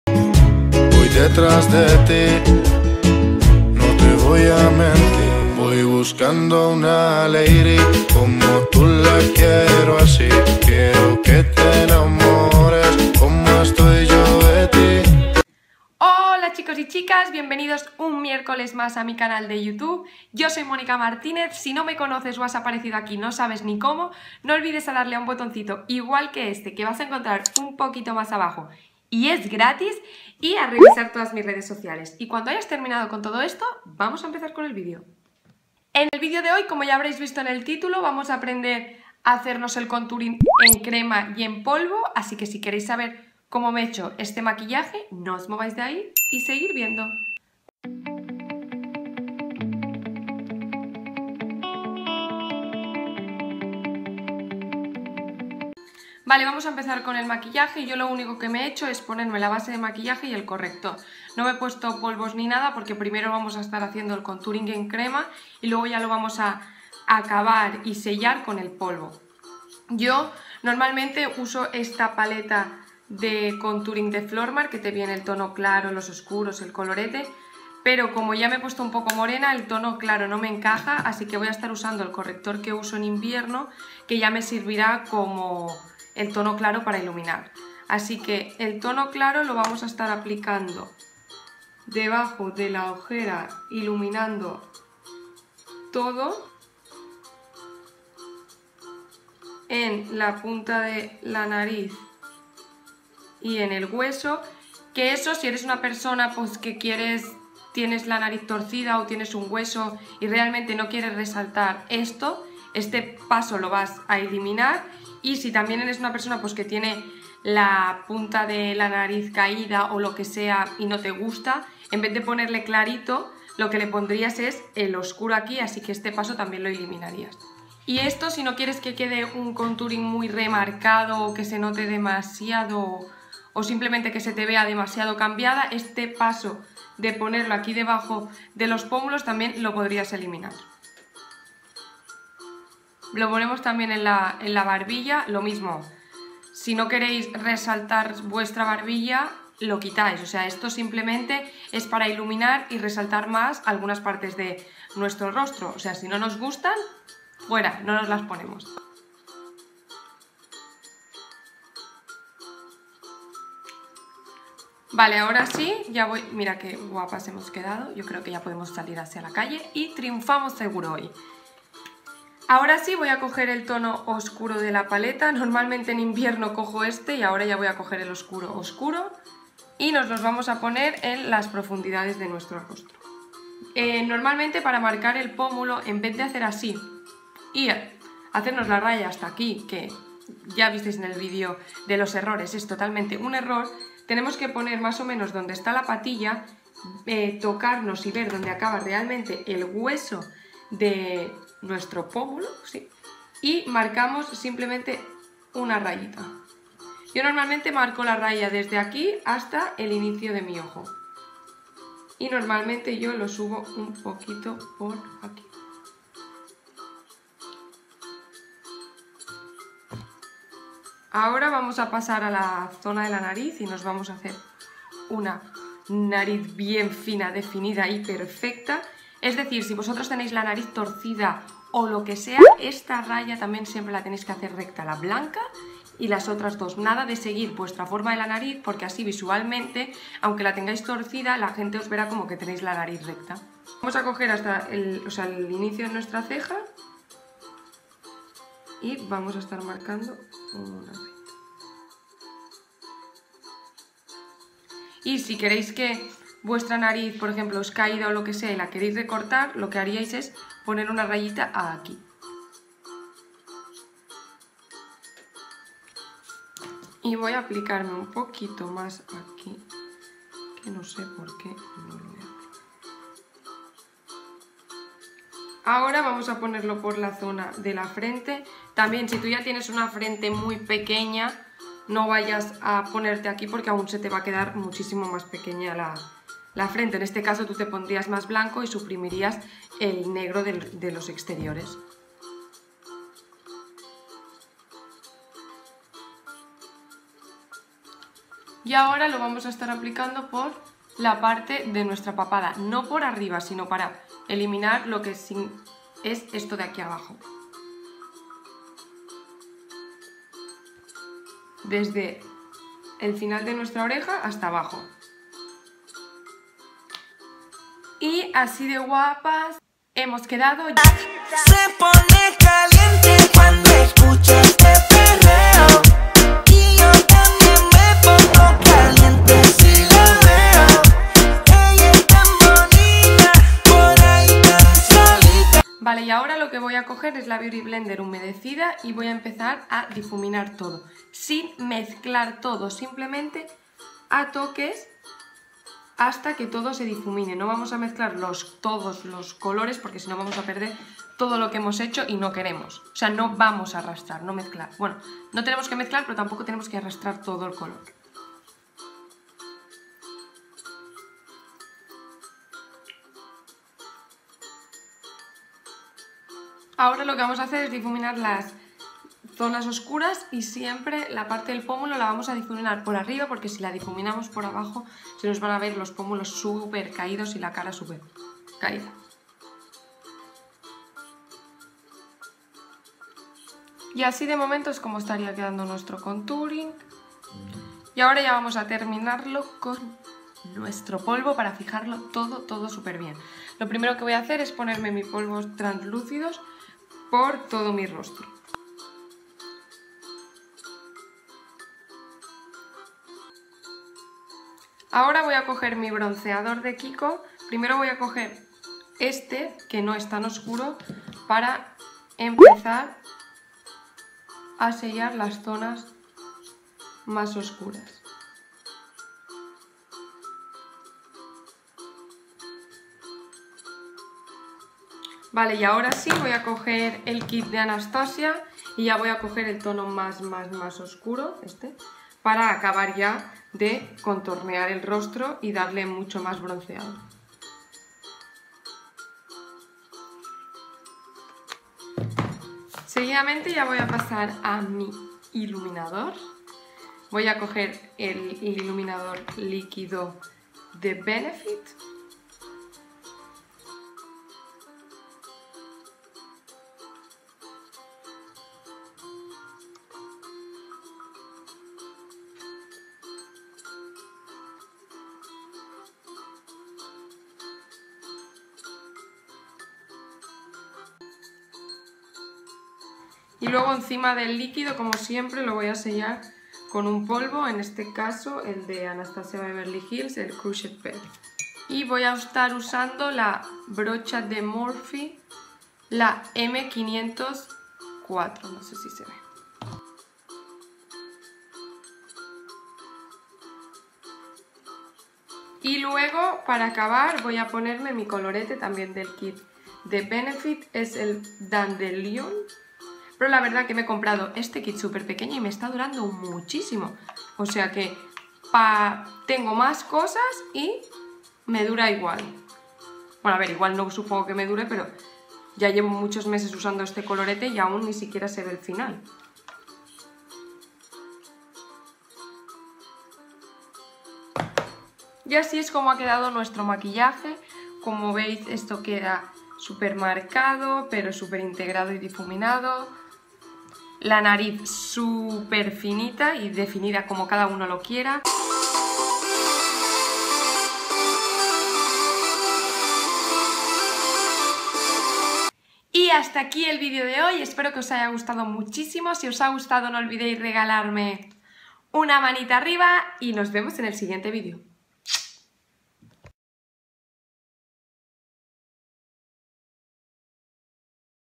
Como Hacer Cirugía Plástica con Maquillaje; Como Contornear el Rostro | Monica Martinez
♡♡ LIKE+COMPARTIR+COMENTAR ♡♡
En el vídeo de hoy vamos aprender como contornear el rostro con maquillaje en crema y polvo, espero que os guste muchooo ♡♡♡ !